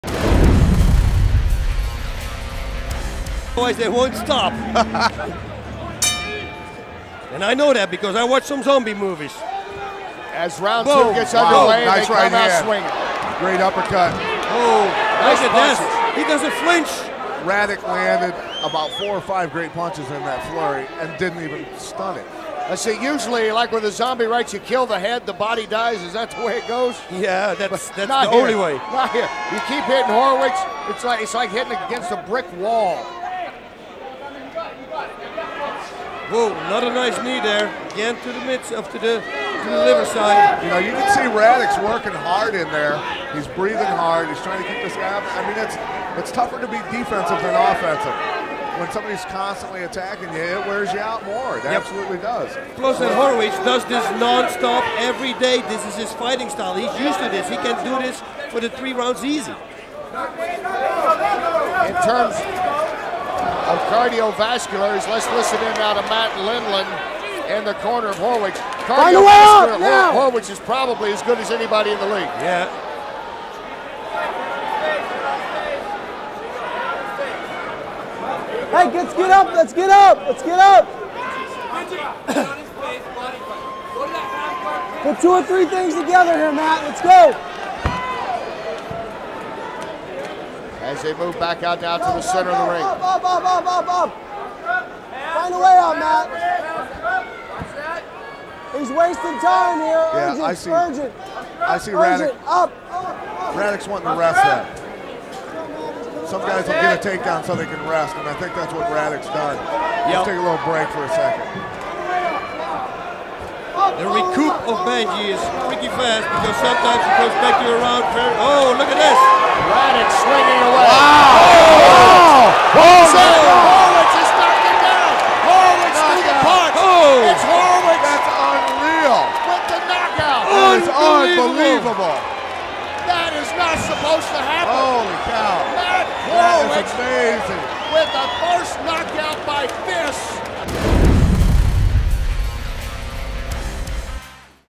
Benji Radach vs. Matt Horwich: Round Two
Horwich again eats a flurry of punches in the process of get